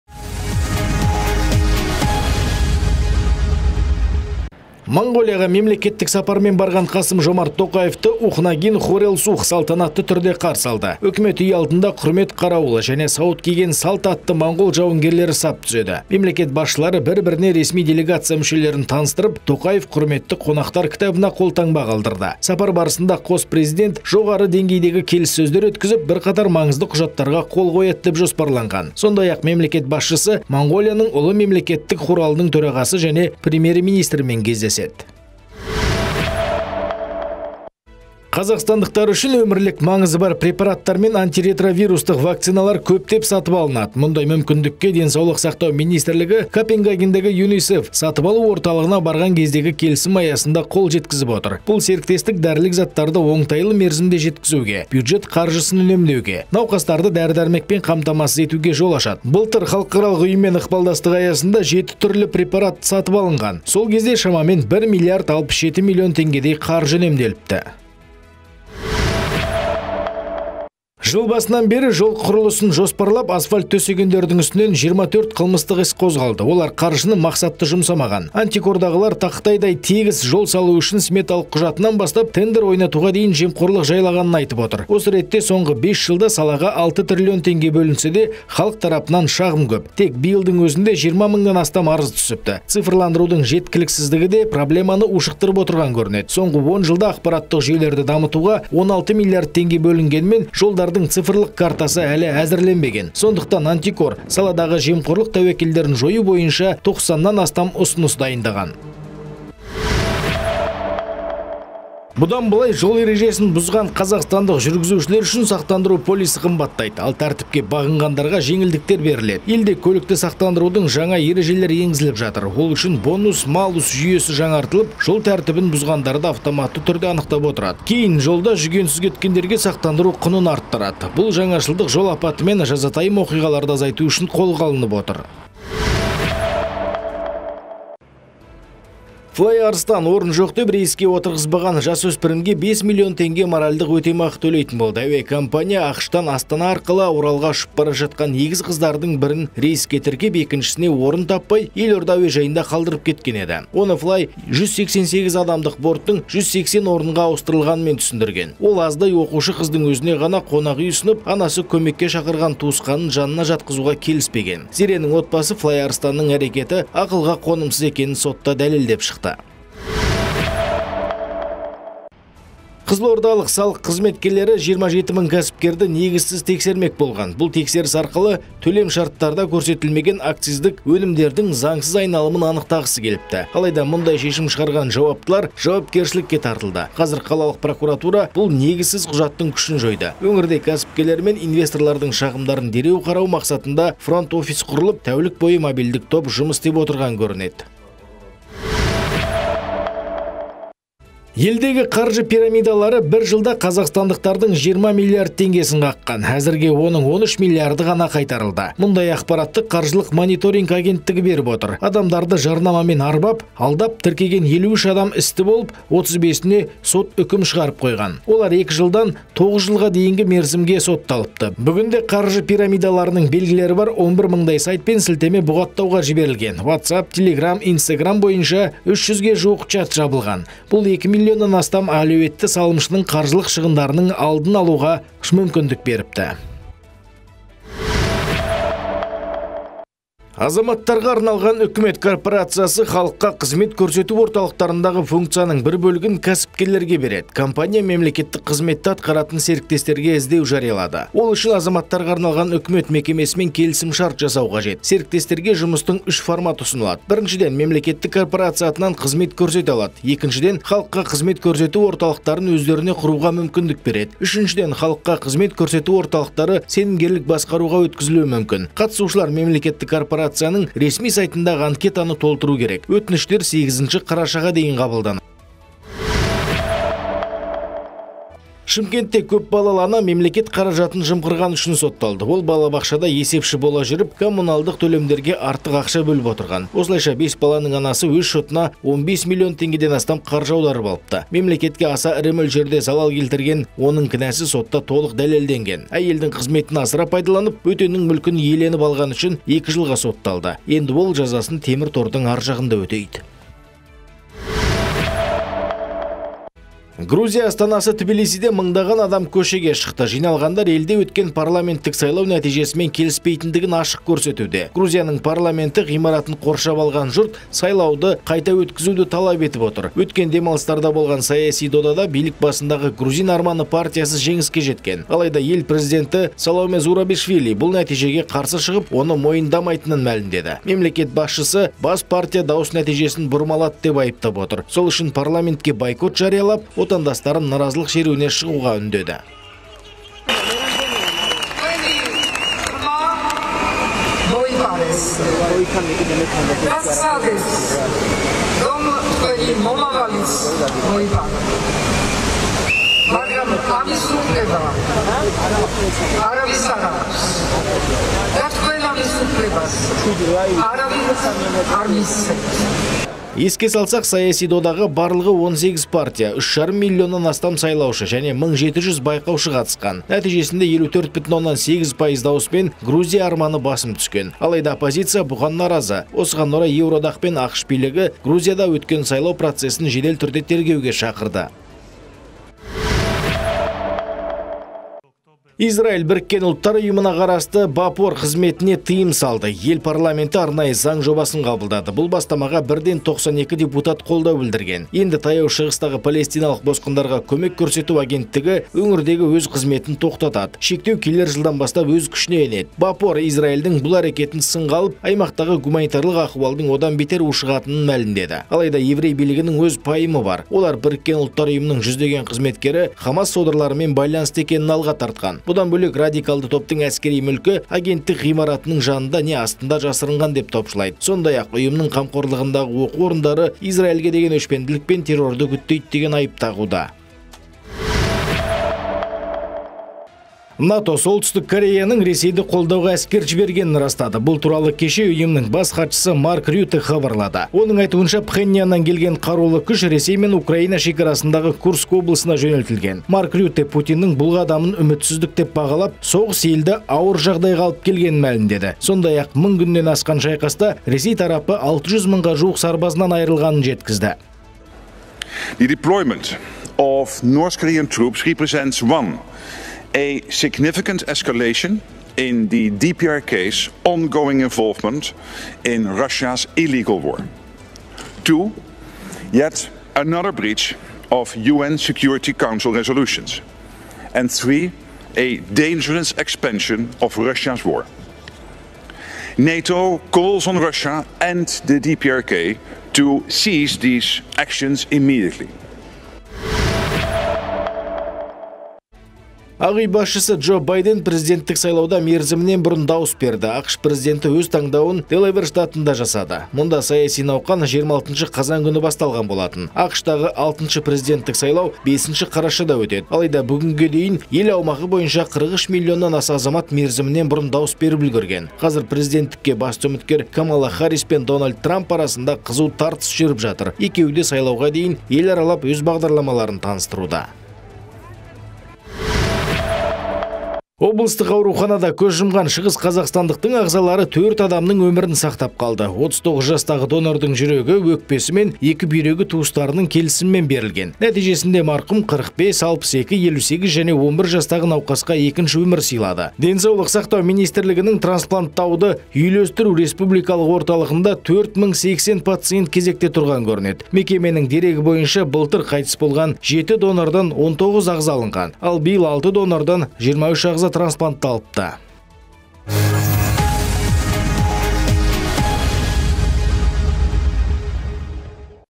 Моңғолия президенті Тоқаевты салтанатты түрде қарсы алды | Қазақстан жаңалықтары
- ЖОЛ ЕРЕЖЕСІН БҰЗҒАНДАРҒА САҚТАНДЫРУ ҚҰНЫ ҚЫМБАТТАЙДЫ
- ЖОЛ ҚҰРЫЛЫСЫН ЖҮРГІЗГЕНДЕРДІҢ ҮСТІНЕН 24 ҚЫЛМЫСТЫҚ ІС ҚОЗҒАЛДЫ
- ҚАЙТЫС БОЛҒАН ШЫҒЫС ҚАЗАҚСТАНДЫҚ 4 АДАМНЫҢ ӨМІРІН САҚТАП ҚАЛДЫ